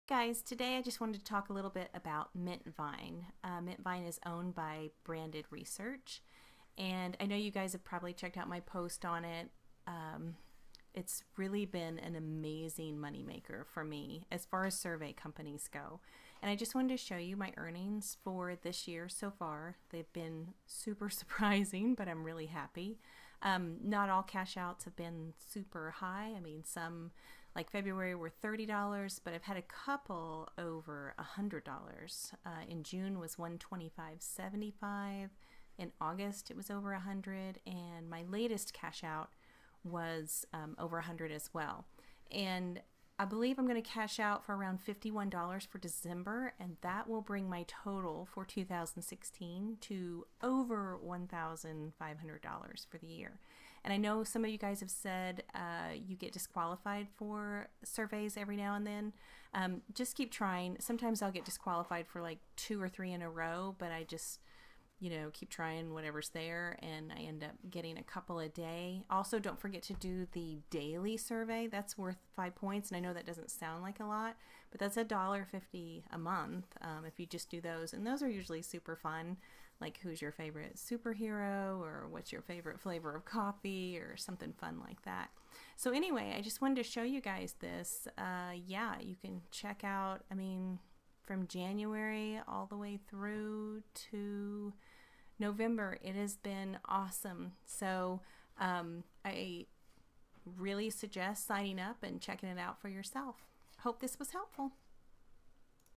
Mintvine (Now Branded Surveys) Earnings Over $1500!
Hey all!
I just wanted to share this quick video regarding Mintvine (now Branded Surveys). A lot of folks have asked if I am still earning -- and I  am so thrilled to say I am! I've earned over $1500 and it's been so easy and amazing.

Update: Mintvine is now Branded Surveys.

WORK AT HOME JOB BOARD

READ FULL POST

GET LEGIT JOB LEADS IN YOUR INBOX

MoneyMakingMommy Work at Home Updates